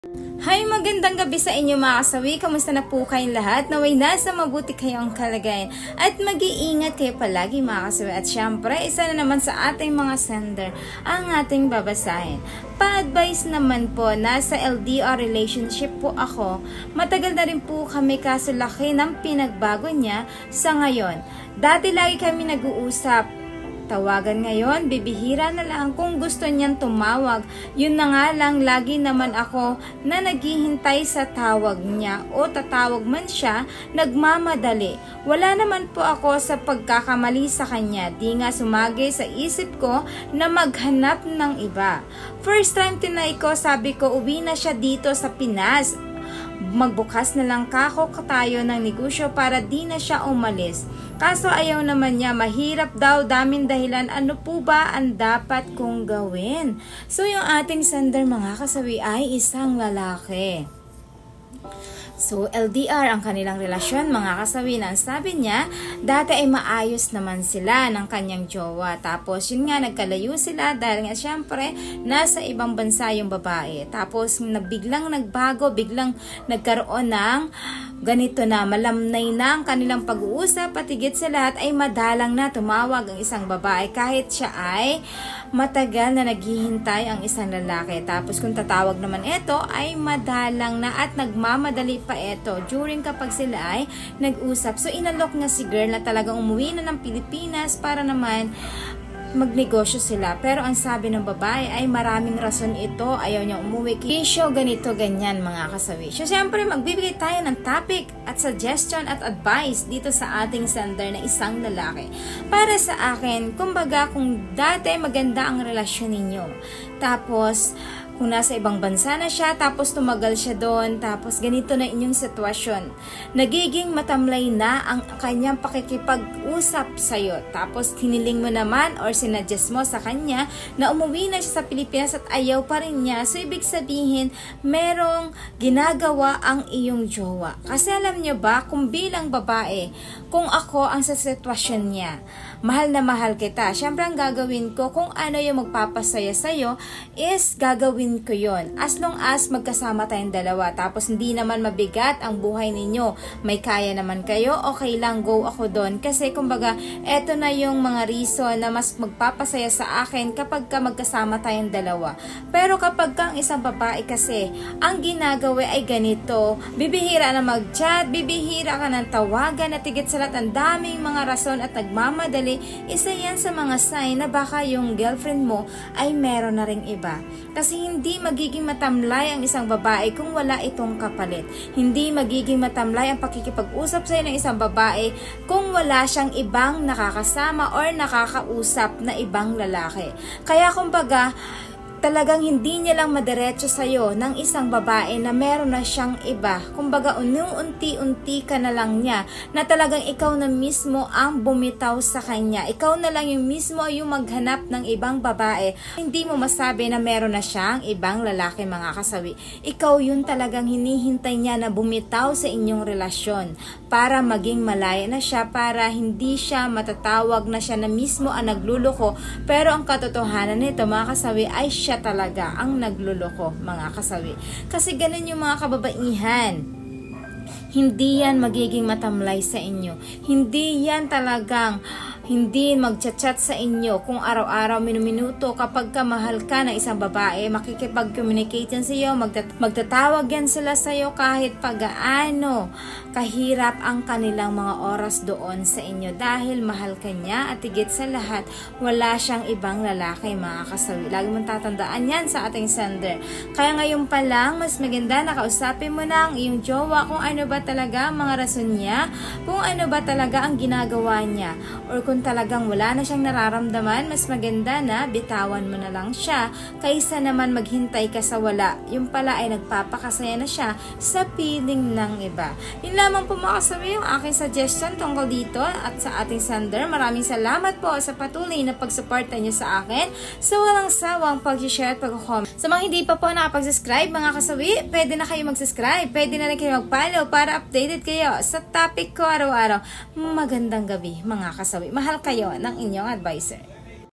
Ito ang MGA RASON kung bakit hindi na masyado nagpaparamdam sayo ang isang BABAE kapag LDR kayo?
Ito ang mga Rason kung bakit hindi nagpaparamdam sayo ang isang BABAE kapag LDR kayo?